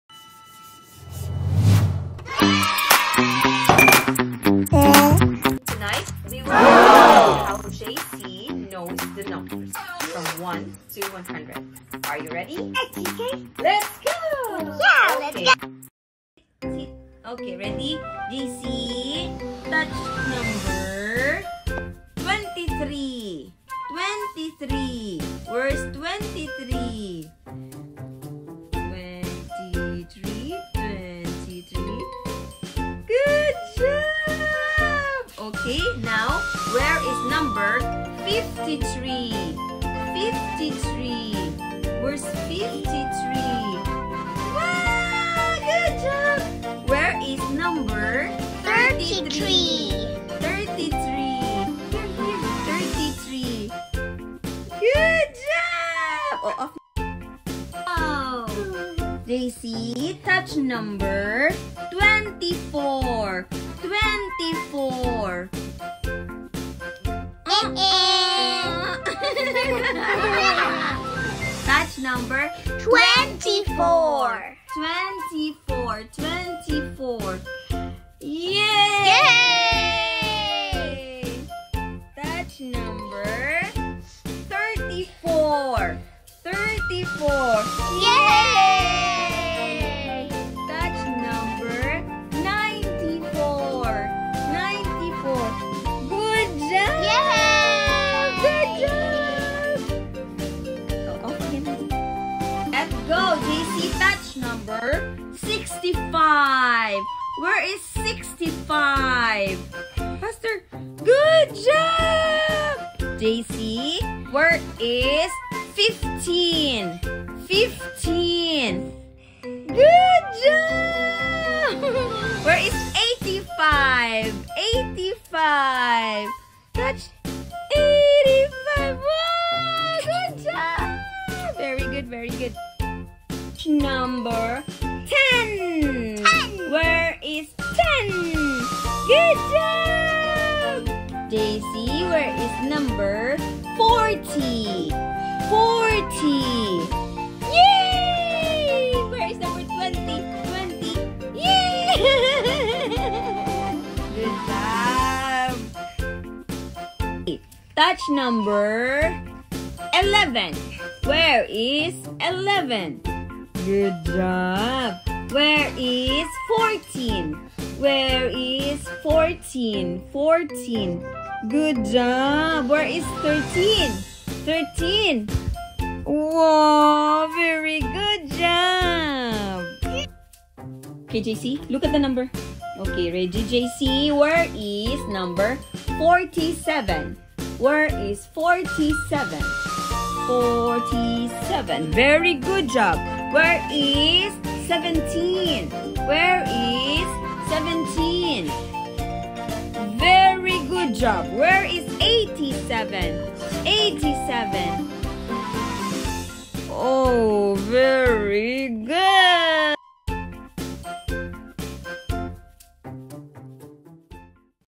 Learning Math is Fun - more than 10 now
Jaycee at 2 years old now, 4 months before he turns 3 years old. He cannot verbally say all numbers and yet he showed signs he knows not only 1-10 but now even till 100.

Here is another test for him. Praise the Lord.